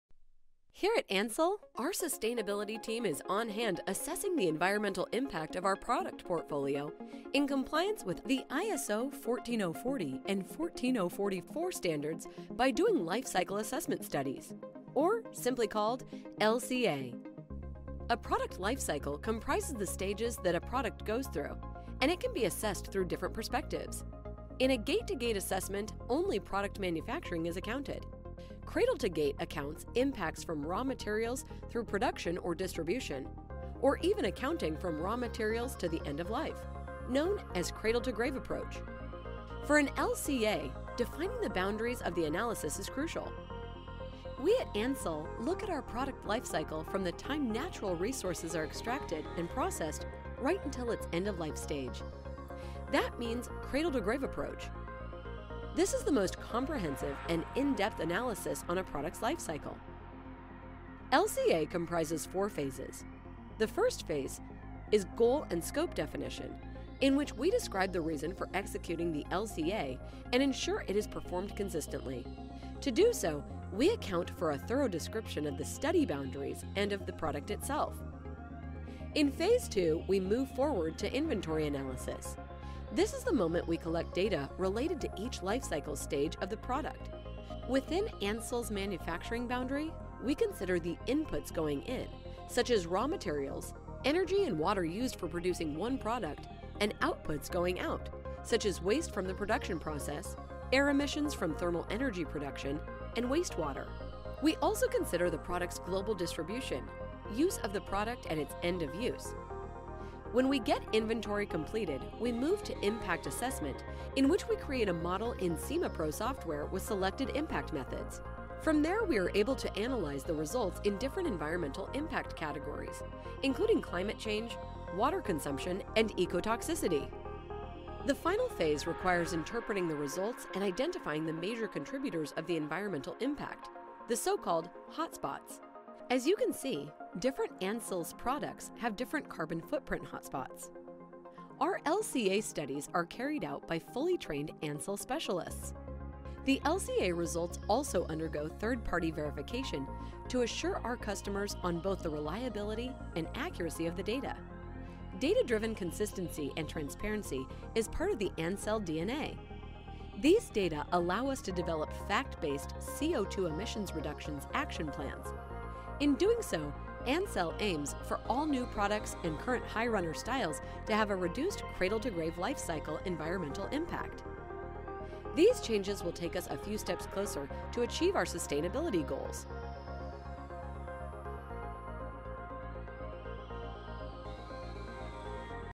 What is a Life Cycle Assessment (LCA)?
Gain insight into Ansell’s Life Cycle Assessment (LCA) as we meticulously evaluate the environmental impact of our product portfolio. Adhering to ISO 14040 and ISO 14044 standards, our process ensures a comprehensive analysis of our product's life cycle, from the time natural resources are extracted until it’s end-of-life.